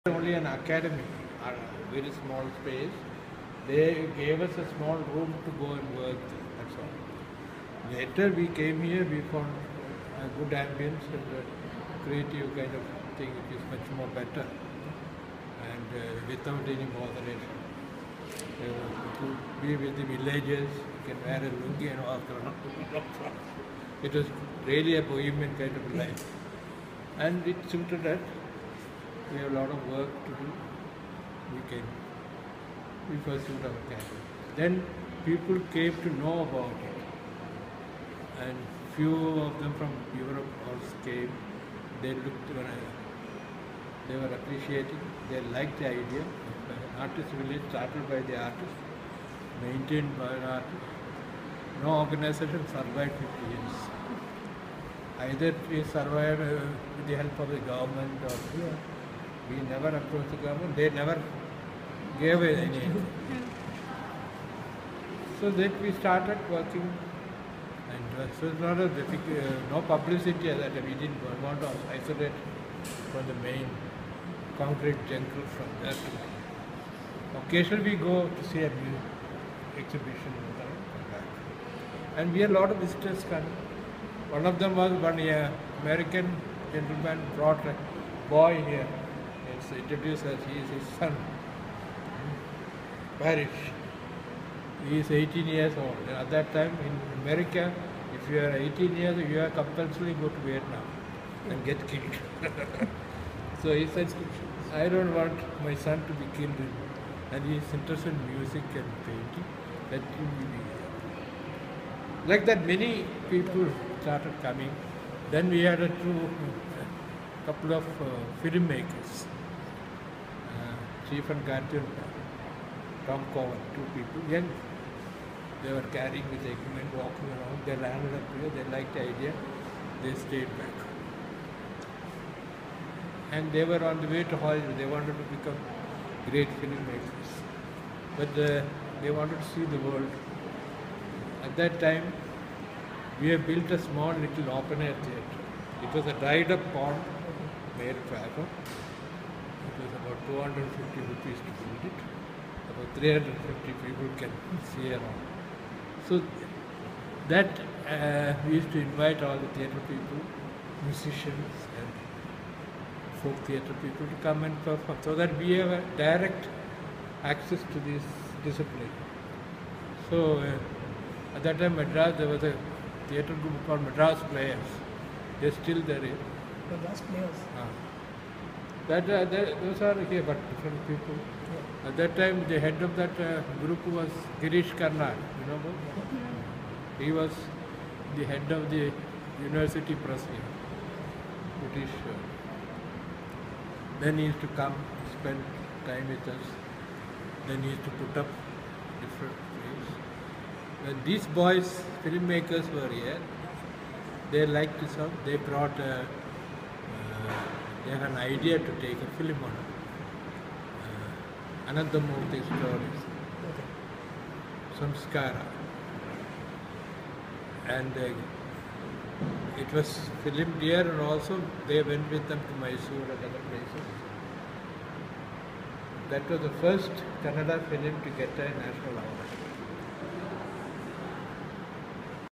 P. Gopinath traces the development of Cholamandalam Artist Village
Guided by K.C.S Paniker , it started as an experiment to promote modern art but soon became India's largest self supporting art colony , with artists from all over the world.